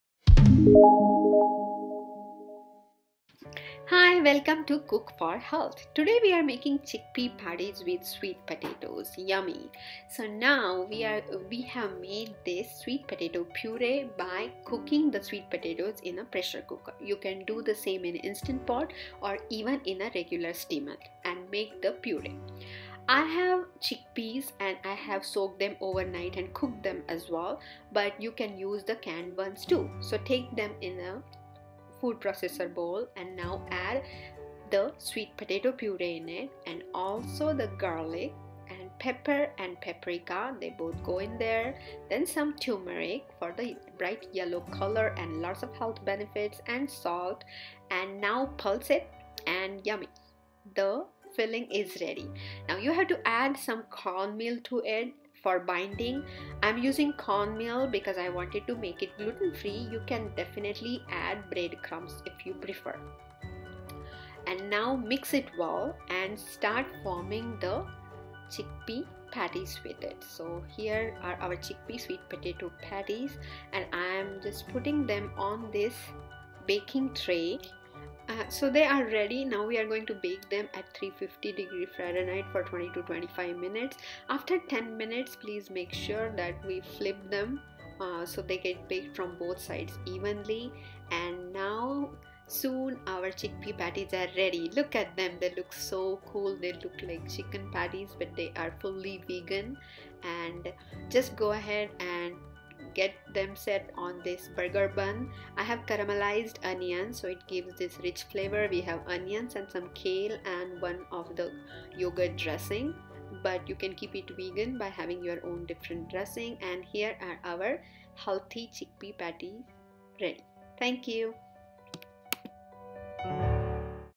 #8 Healthy (Plant Based) Burger Patty!!!!
#8 Healthiest Chickpea burger Patty

Benefits and ingredients:
Chickpeas Benefits:
• Improves Colon Health
• Aids weight management
• They help with digestion
• Lowers cholesterol
• Boosts immunity
• Builds muscle
• Regulates blood sugar
• Great protein source

Ingredients:
• 3 cups chickpeas, soaked & cooked or canned (2 cans)
• ½ c sweet potato puree
• 2 garlic cloves
• ¼ tsp cayenne
• ¼ tsp paprika
• ¼ tsp turmeric
• ½ tsp salt